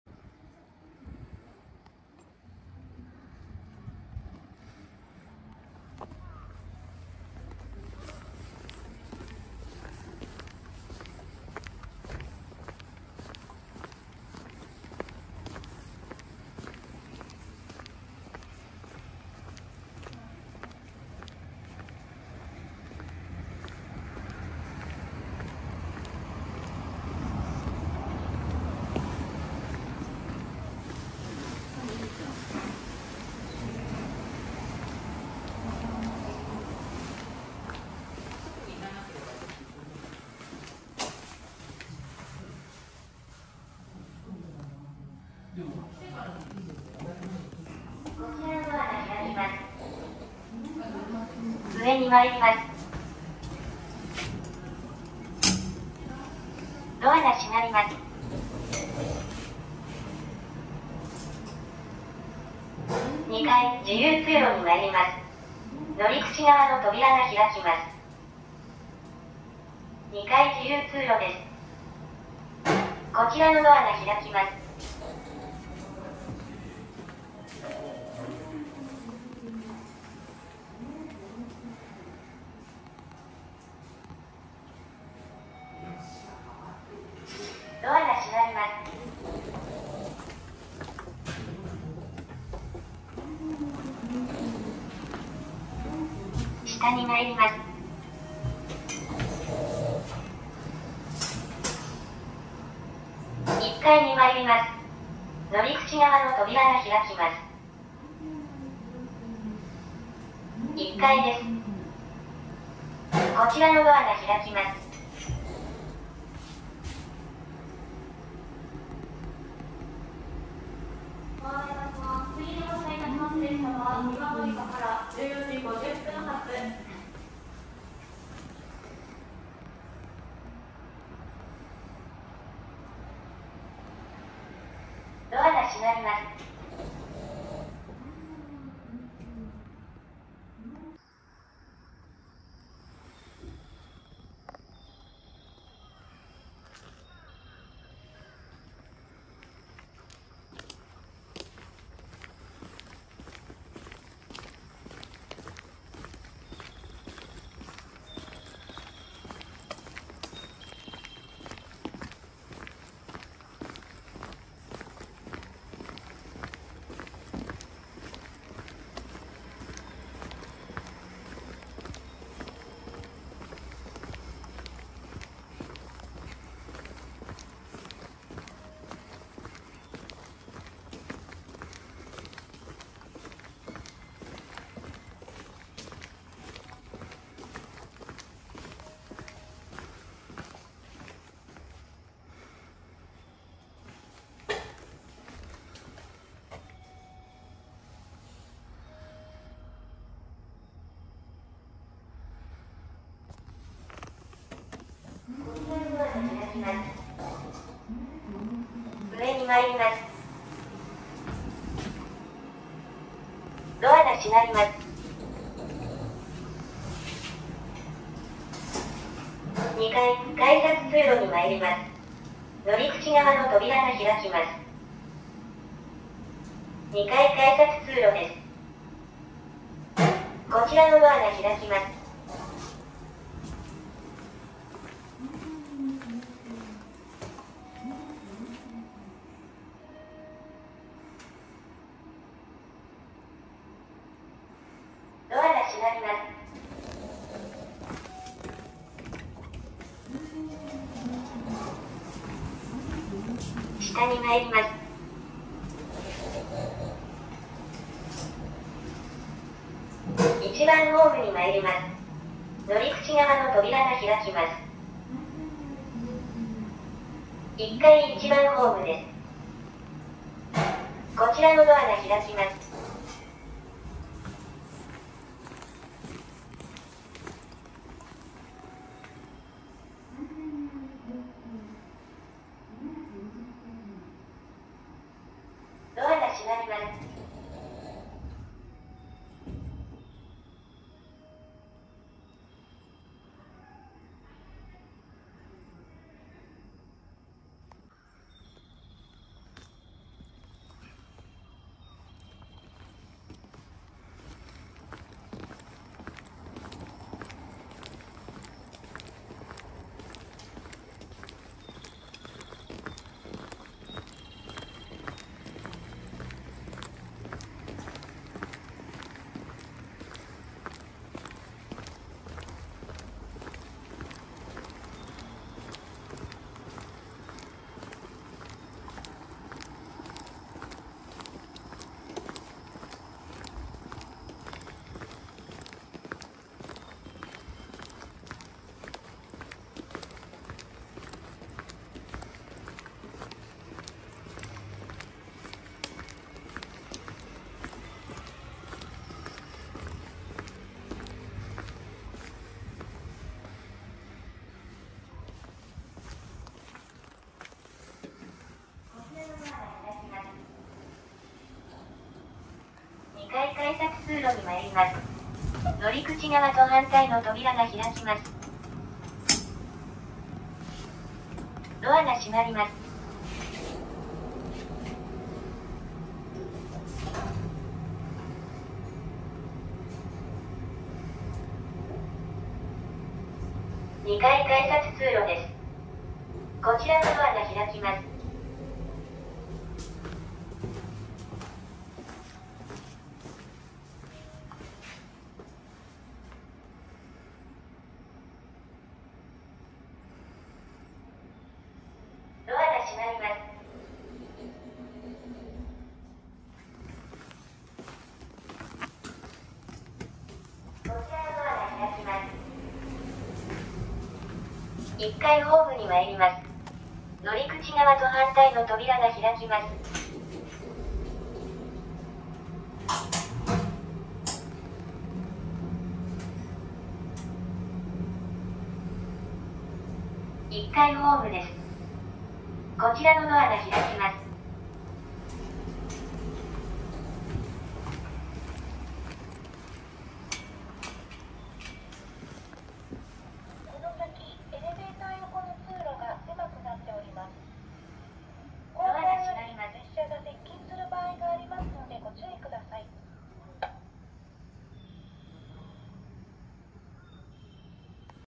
[紀伊勝浦駅]東芝製エレベーター 駅前広場⇔コンコース→ホーム階⇔改札階 2基まとめ
紀伊勝浦駅の昇降機はエレベーターのみ2基設置。全部東芝製。エスカレーターは無し。

1基目。駅前広場⇔コンコースを連絡するが、これは所謂1台2役の入口選択型エレベーター。駅前広場から乗った場合は必ずコンコースへ、①ホームから乗った場合は必ず改札階へと連絡する。つまり、出口は指定できない構造。

①ホーム⇔改札階の連絡の場合、開かない反対側の扉を見ると、駅前広場とホームの高さが違うのが面白い。

2基目。②③ホーム⇔改札階を連絡。こちらは通常の通り抜けタイプ。